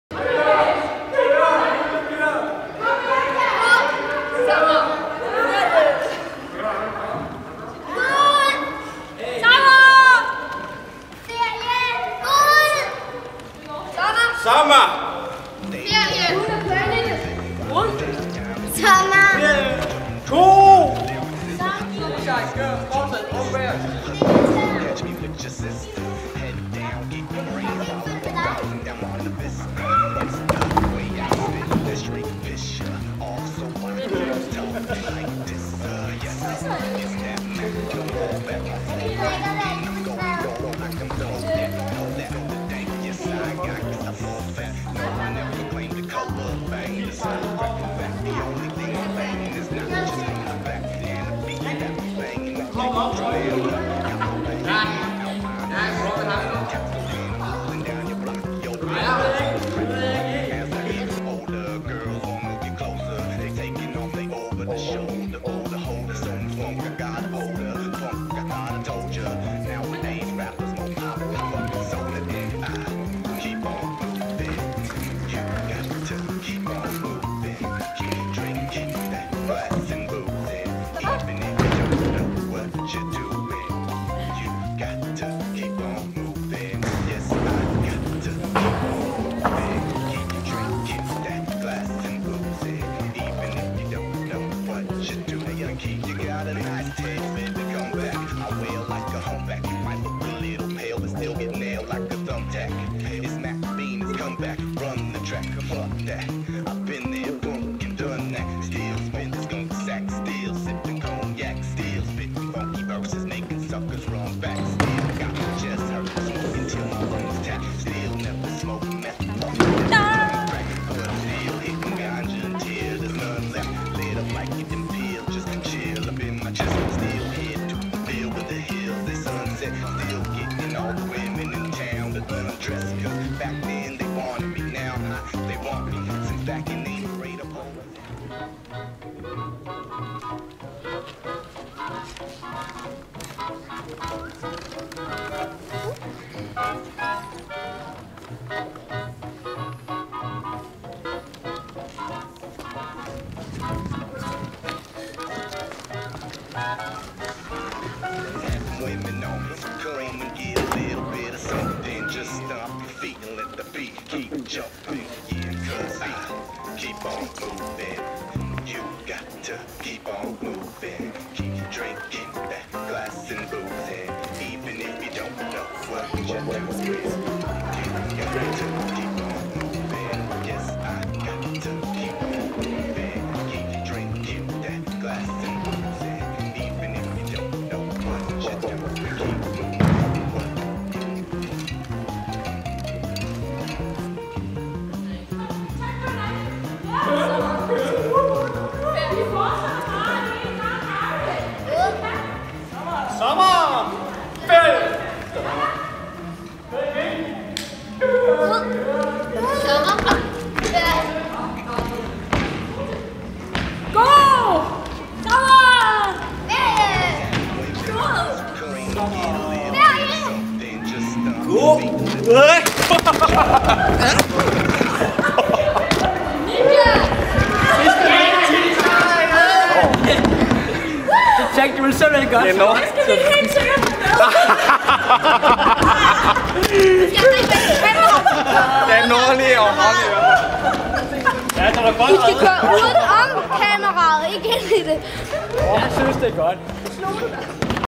God sommer ferie 2014 - SIFU - Stenlille Unicyklister
God sommerferie til alle fra Stenlille unicyklister 2014.
Se lidt af det vi har lært.
Unicykling er sjovt og alle kan være med.